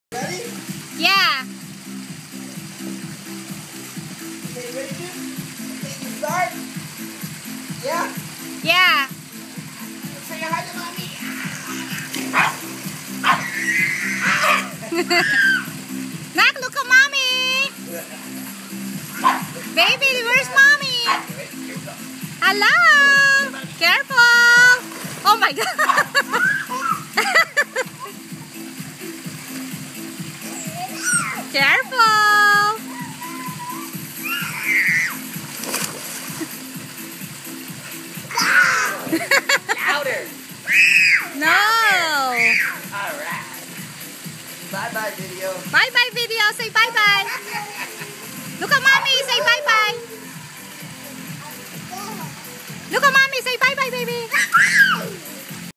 Daddy Tossing Baby In The Pool
May 13, 2013 - My 2 boys are having fun in the pool.

Stop the hate. BE KIND. Support and Inspire each other :)